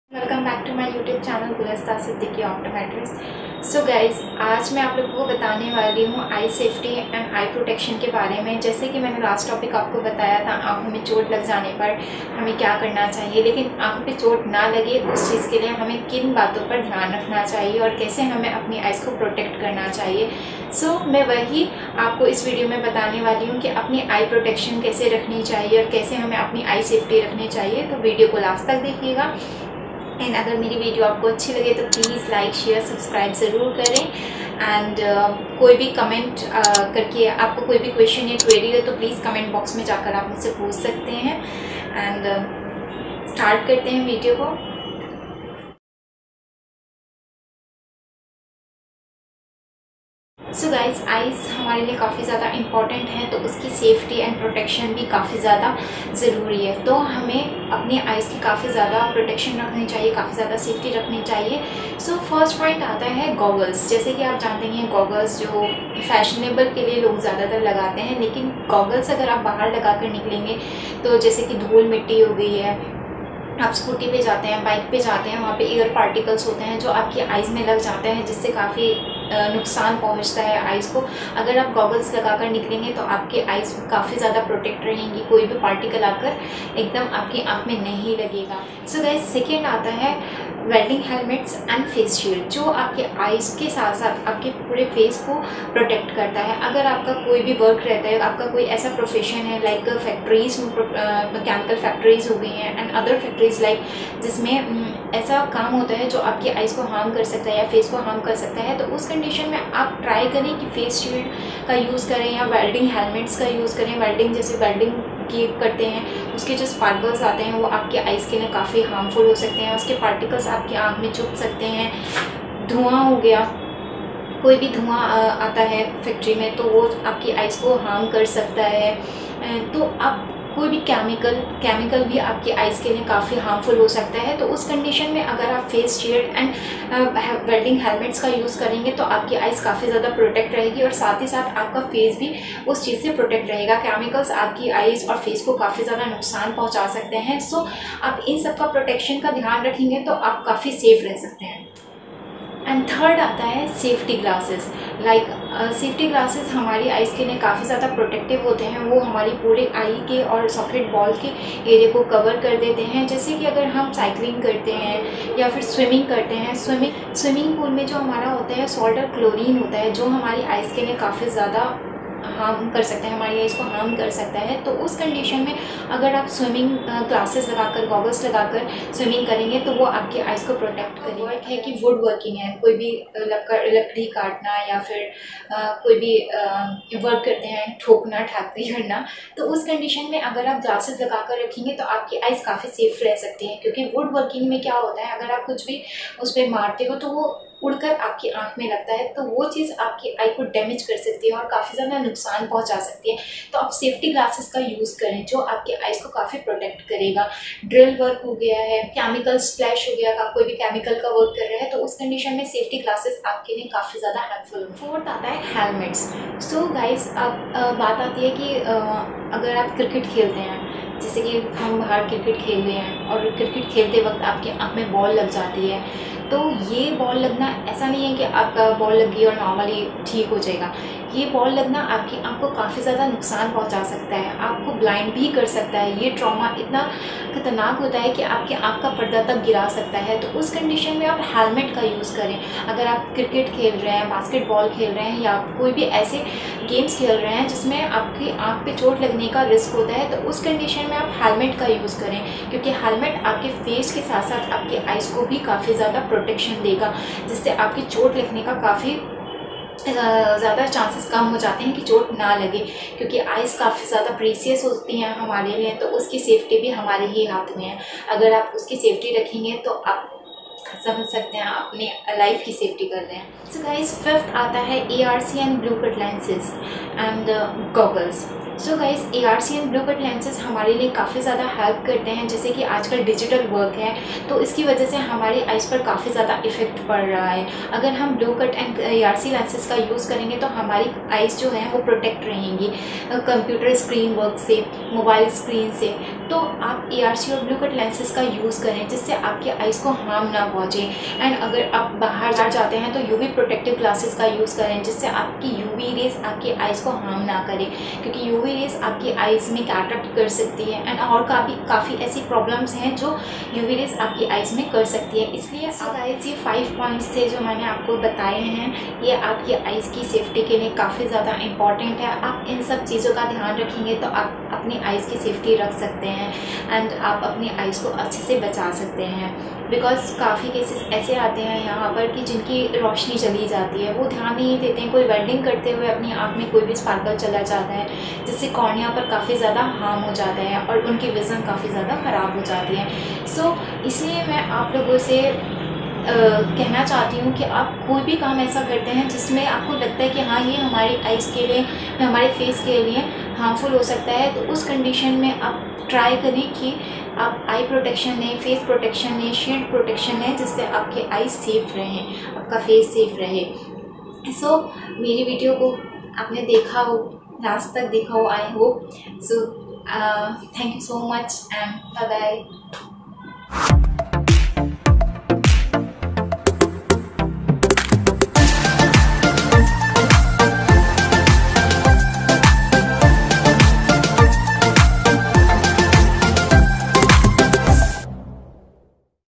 EYE SAFETY AND EYE PROTECTION
Eye protection is protective gear for the eyes, and sometimes face, designed to reduce the risk of injury. Examples of risks requiring eye protection can include: impact from particles or debris, light or radiation, wind blast, heat, sea spray or impact from some type of ball or puck used in sports.
Eye protection are typically separated into categories based on the style of eye wear and the hazard they are designed to reduce. There categories include: Spectacles with side protection; Goggles; Welding helmet; Welding Hand Shields; Non-Rigid Helmets (hoods); Face shield; and Respirator Face pieces.

and this is my contant every words and everything is done by me
video editing done by me
topic idea done by me
no copyright